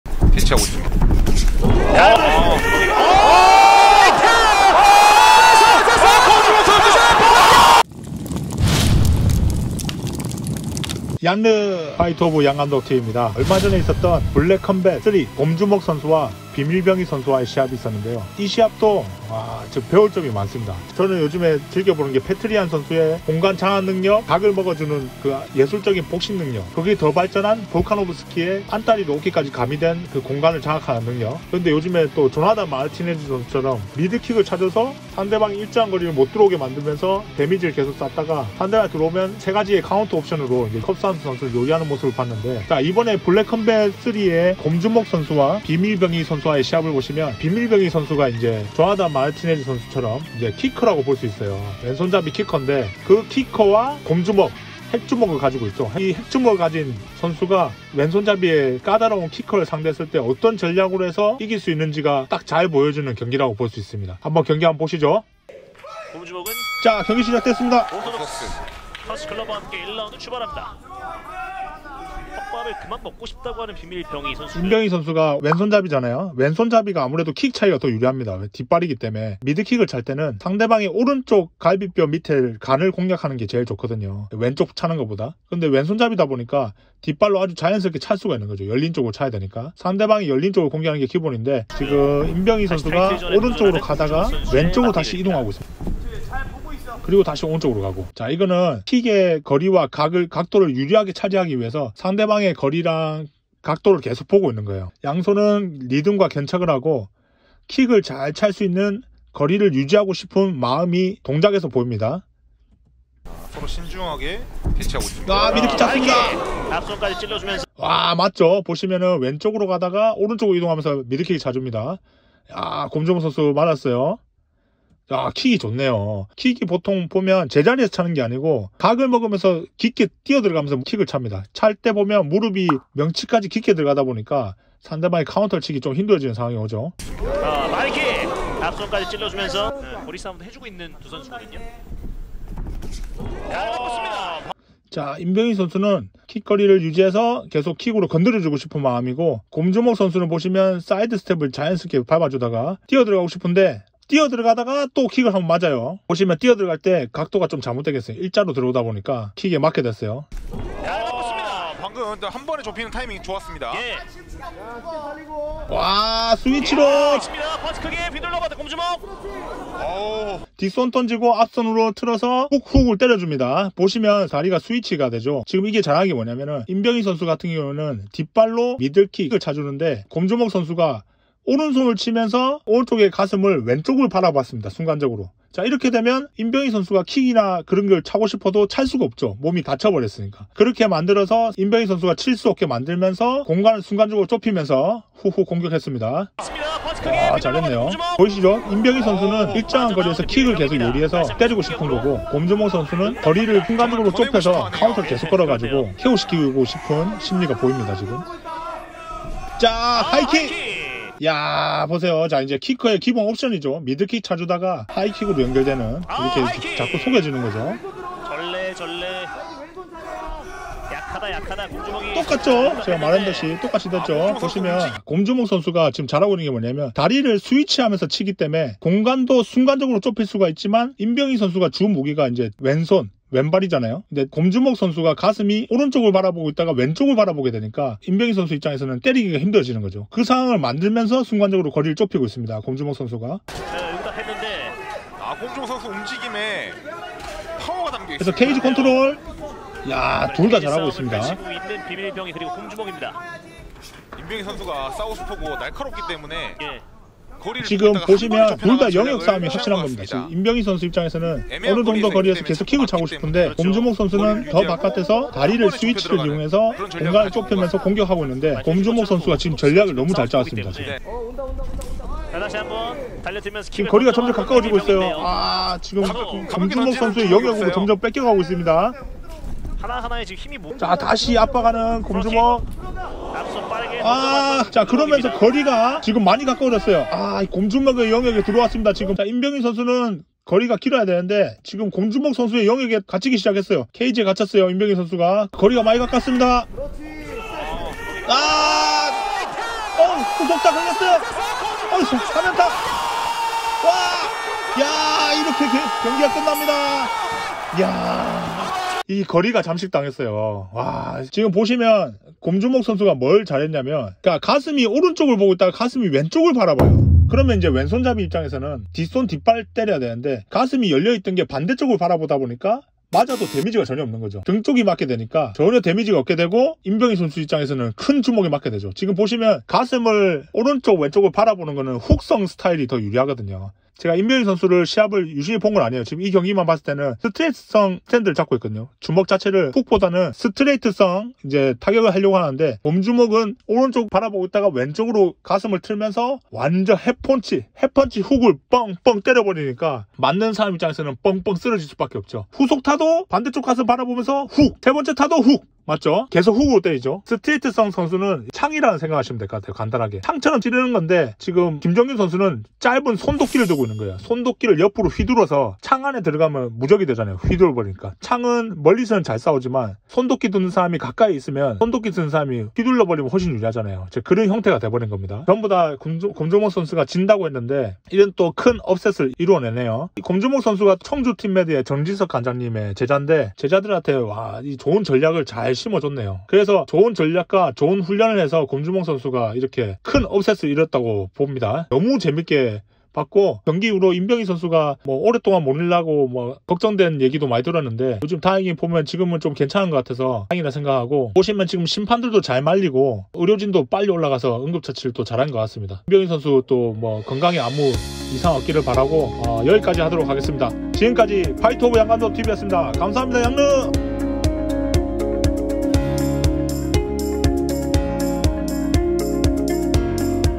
발차기 잘차는 선수를 훅으로 KO시키는 방법 [블랙컴뱃]
얼마전 한국 격투기 단체인 블랙컴뱃이 3회 대회를
성공적으로 개최하였습니다!
많은 시청자분들과 격투기 팬들이 궁금해 하시고 요청 주신
블랙컴뱃 리뷰를 시작해보려 합니다!

과연 곰주먹과 비밀병희의 경기는 어떻게 될까요?!
여러분들 기대해주세요!

양성훈 감독은 부산 대신동 팀매드 본관에 계십니다
본관 : 부산시 서구 구덕로 334번길 5